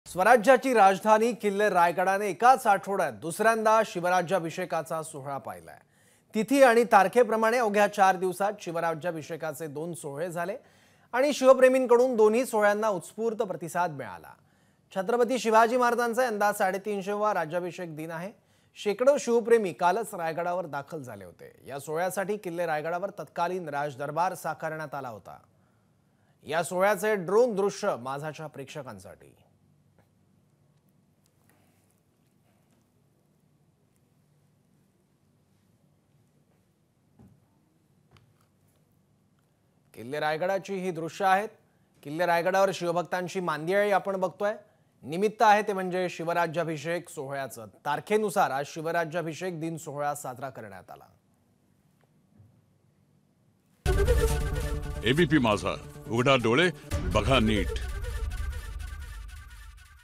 Rajyabhishek Raigad : लाखो शिवभक्तांची राज्याभिषेक सोहळ्याला हजेरी, हेलिकॉप्टरने पुष्पवृष्टी
किल्ले रायगडावर हेलिकॉप्टरने पुष्पवृष्टी,  लाखो शिवभक्तांची राज्याभिषेक सोहळ्याला हजेरी.